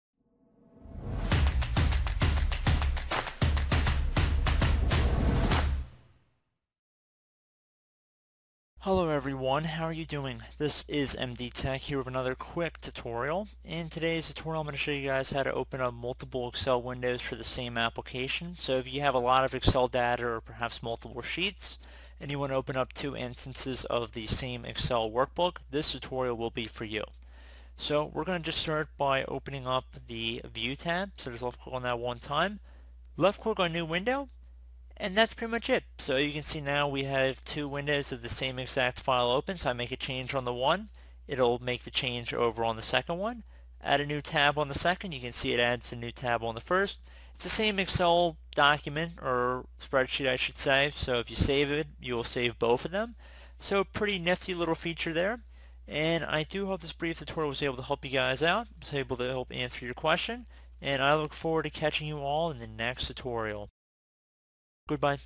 Open One Excel Workbook Multiple Times For Side By Side Use
You can quickly compare two worksheets in the same workbook or in different workbooks by viewing them side by side. You can also arrange multiple worksheets to view them all at the same time.